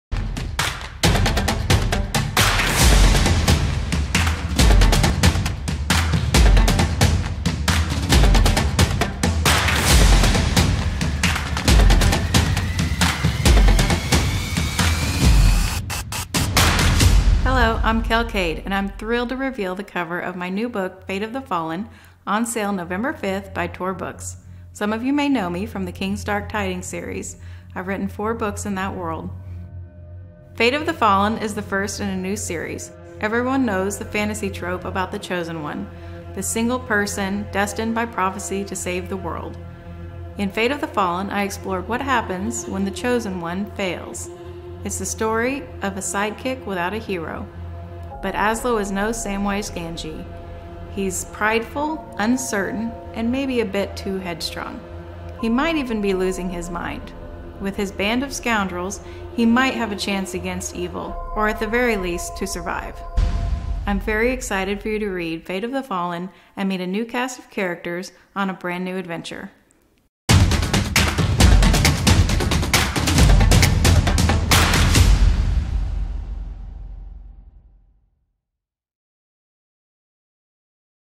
Fate of the Fallen by Kel Kade
Fate of the Fallen is the start of a brand new adventure from New York Times bestselling author Kel Kade

Not all stories have happy endings.

Everyone loves Mathias. Naturally, when he discovers it’s his destiny to save the world, he dives in head first, pulling his best friend Aaslo along for the ride.

However, saving the world isn’t as easy, or exciting, as it sounds in the stories. The going gets rough and folks start to believe their best chance for survival is to surrender to the forces of evil, which isn’t how the prophecy goes. At all. As the list of allies grows thin, and the friends find themselves staring death in the face they must decide how to become the heroes they were destined to be or, failing that, how to survive